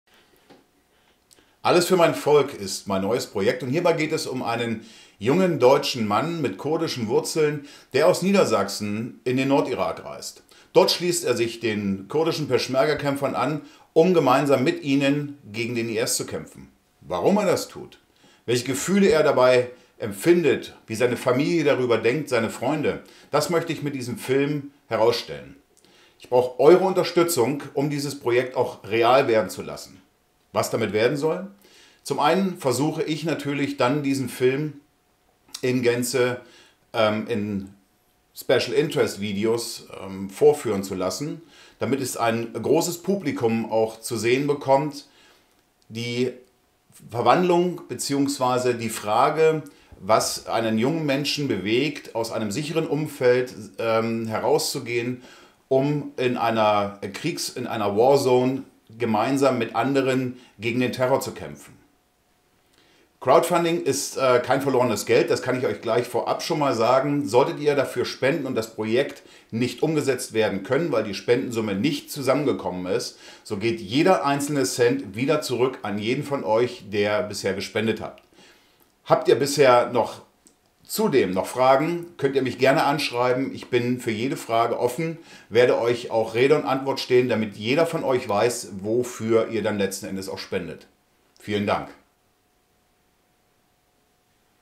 Alles für mein Volk | Crowdfunding
Ein junger Deutscher mit kurdischen Wurzeln reist aus Niedersachen in den Irak, um dort an der Seite der Peschmerga gegen den Terror des IS zu kämpfen.

und ich würde mich natürlich freuen, wenn auch ihr dabei seid, um mich bei meiner Recherche und der anstehenden Reise in den Irak zu unterstützen.